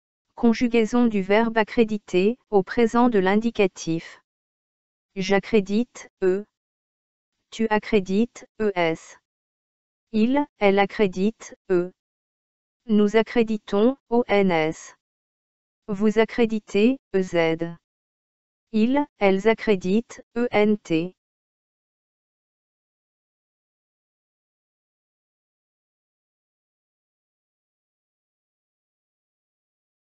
Accréditer | Présent de l'indicatif
Conjugaison du verbe Accréditer, au présent de l'indicatif.
j'accrédite
tu accrédites
il, elle accrédite
nous accréditons
vous accréditez
ils, elles accréditent